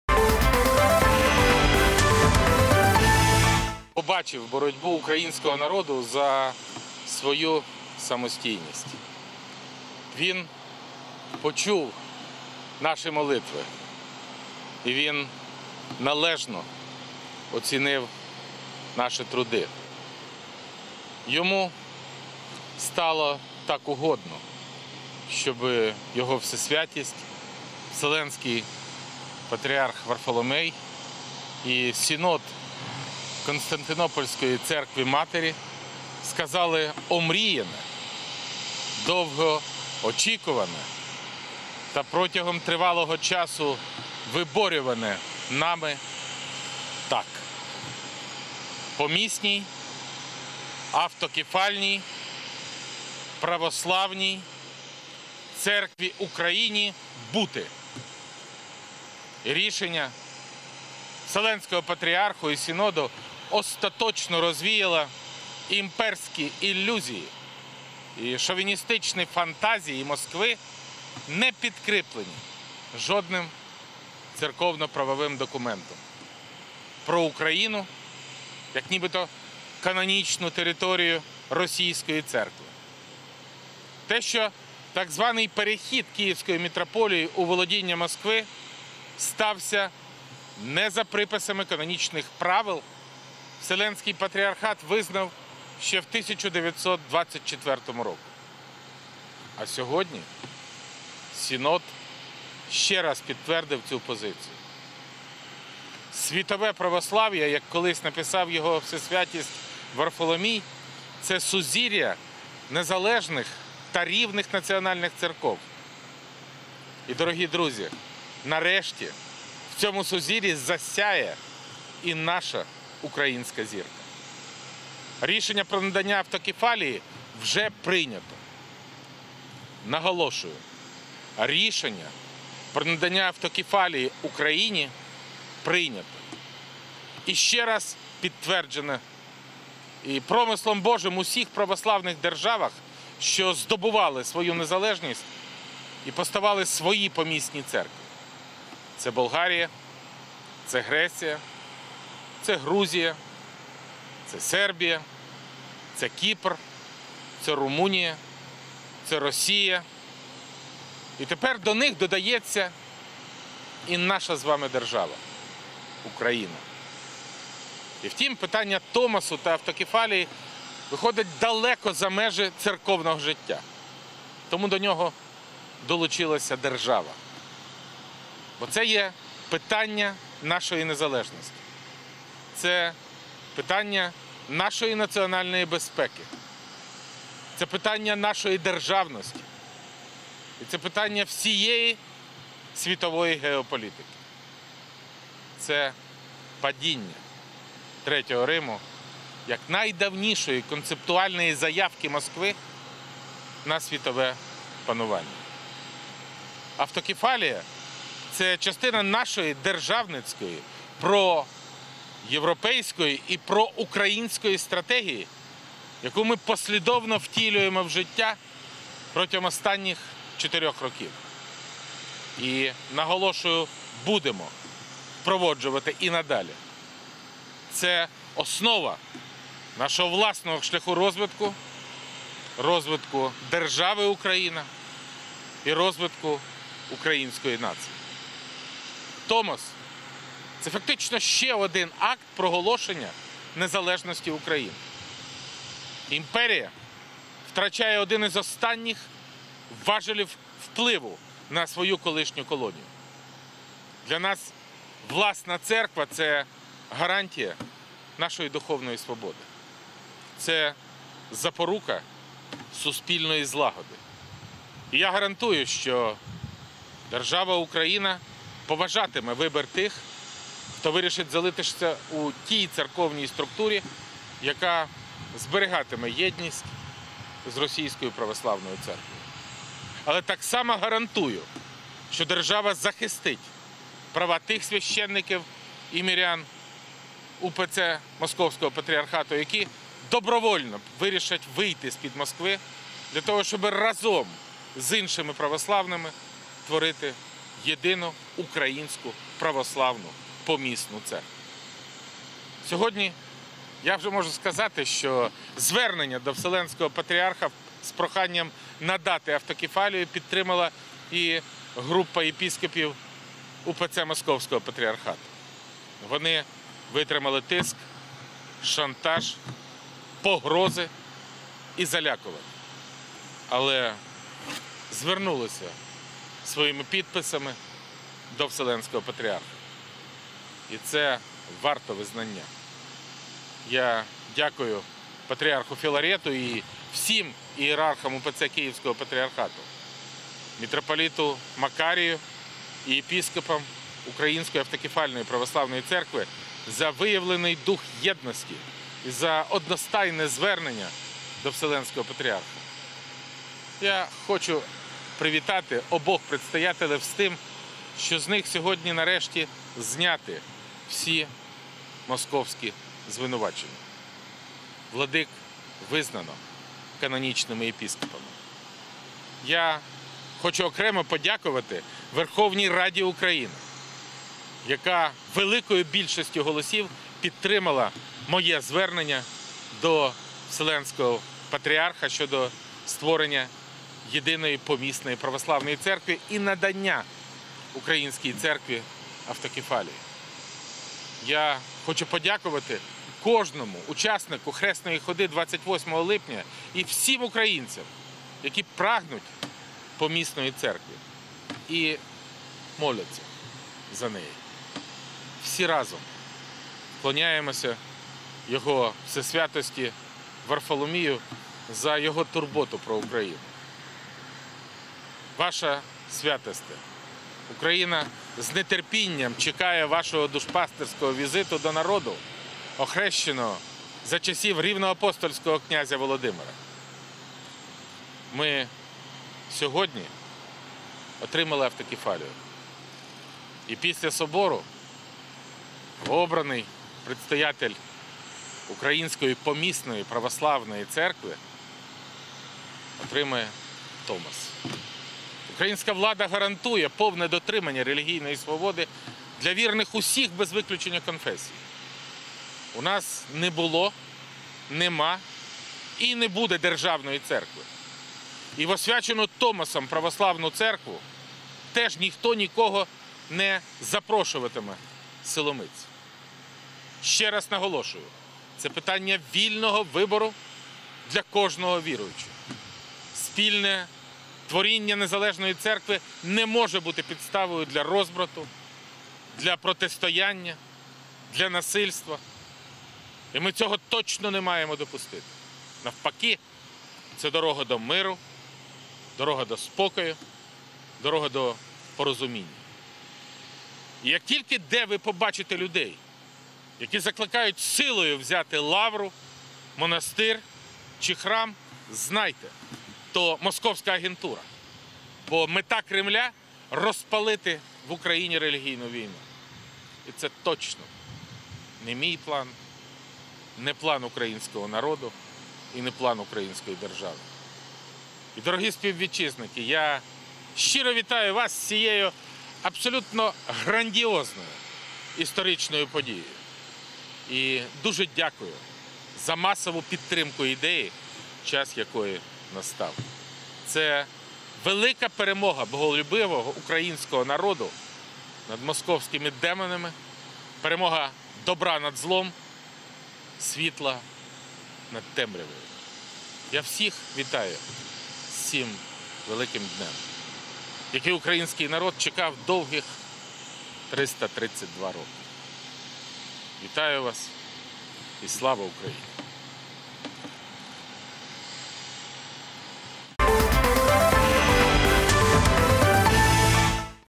Петр Порошенко поздравил украинцев с решением Синода предоставить Томос об автокефалии Украине
Синод Вселенского патриархата принял решение предоставить Томос об автокефалии православной церкви в Украине.